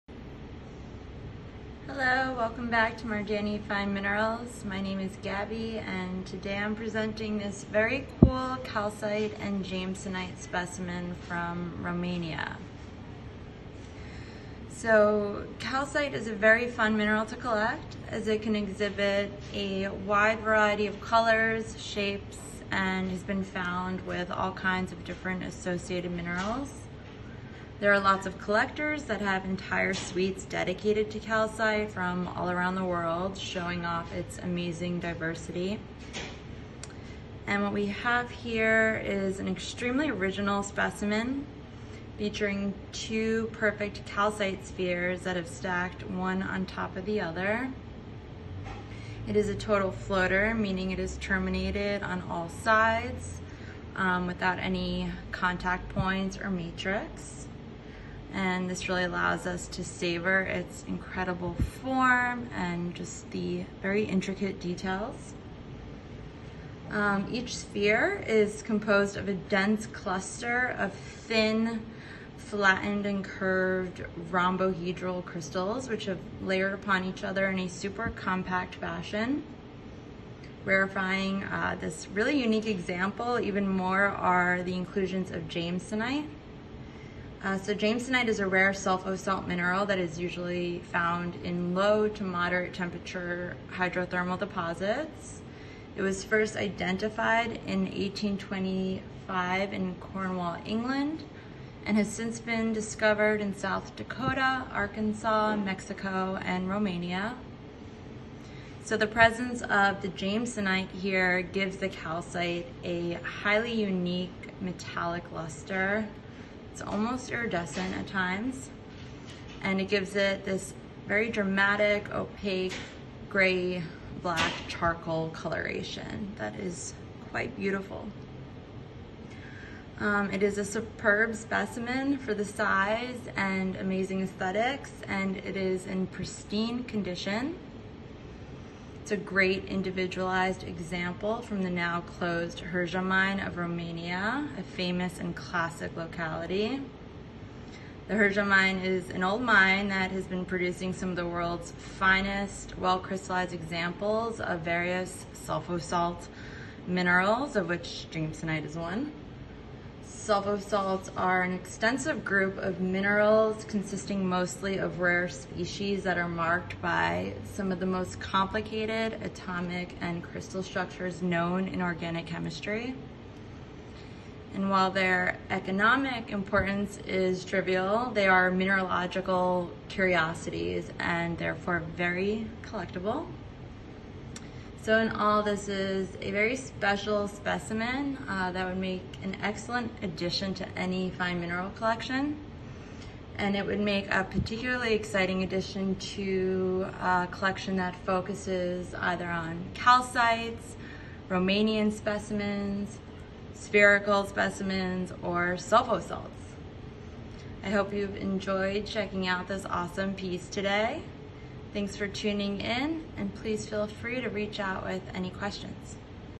Mardani Fine Minerals: Calcite & Jamesonite # 17092
Gabby Lipton presents this intriguing calcite & jamesonite specimen from Romania. Sound on to learn more🔊

Herja Mine, Chiuzbaia, Baia Mare, Maramures Co., Romania
Dimensions: H: 5.5 cm   W: 3 cm   D: 3.2 cm   Wt: 92 g